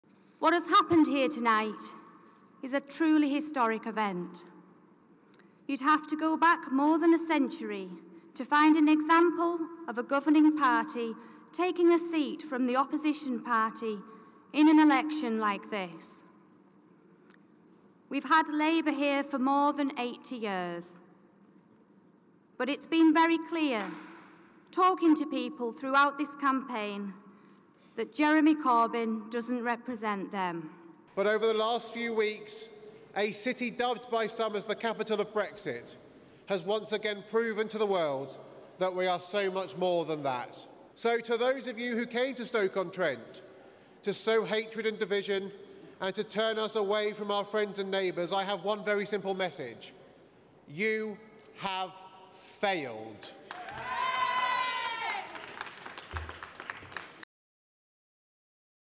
Copeland and Stoke by-election victory speeches
Labour loses Copeland, a heartland seat dominated by the party since 1924.
Conservative Trudy Harrison took 13,748 votes in the byelection to Labour’s 11,601. Half an hour before, the party’s Gareth Snell saw off Ukip leader Paul Nuttall in a bitter battle for Stoke Central.